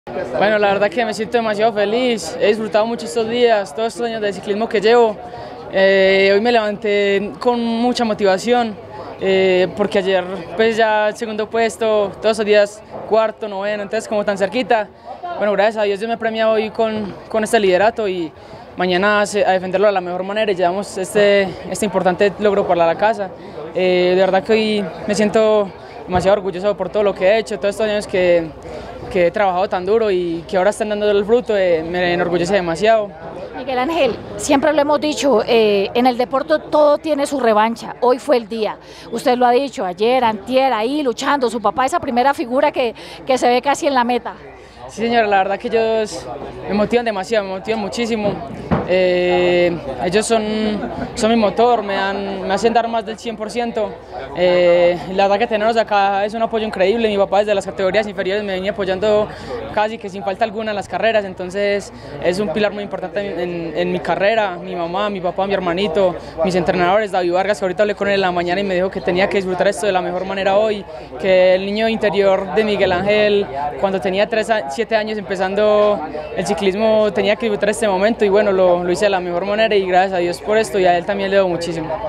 Miguel Ángel Marín líder general Vuelta del Porvenir
Imágenes etapa 4 del Tour Femenino y Vuelta del Porvenir
Cordial saludo, compartimos el material en video de la cuarta etapa del Tour Femenino y Vuelta del Porvenir.

*Vuelta del Porvenir y Tour Femenino 2024*

13.4 KM
Etapa 4️⃣ CRI

🚴🏻🚴🏻‍♀️ Corrales - Floresta

♦️Ganadora Etapa y Líder de la Clasificación por Puntos:
🎙️Estefanía Herrera:
➡️ Mujeres Antioquia

♦️Líder de los Sprints Especiales:
🎙️Paula Andrea Carrasco
➡️ Colombia Potencia de la vida

♦️Líder de la Clasificación General, Líder de la Montaña:
🎙️Laura Daniela Rojas
➡️ Boyacá es para Vivirla

♦️Ganador Etapa y Líder de los Premios de Montaña:
🎙️Jerónimo Calderón
➡️Team Sistecrédito

♦️Líder de la Clasificación General y Líder de la Clasificación por Puntos:
🎙️Miguel Ángel Marín
Avinal Alc Carmen de Viboral

🎙️Alejandro Peña
➡️Colombia Potencia va  de la vida